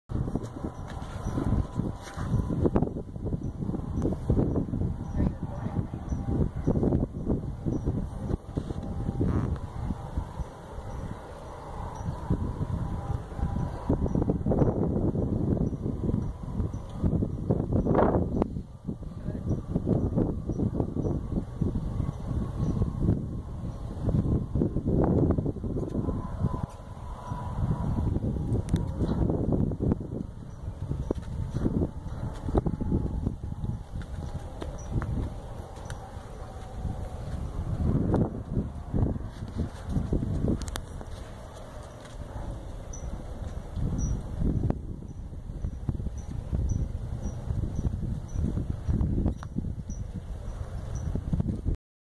Swyper rope work
Swyper on her 8th ride doing rope work for the first time. She did awesome! All following our WRR training program.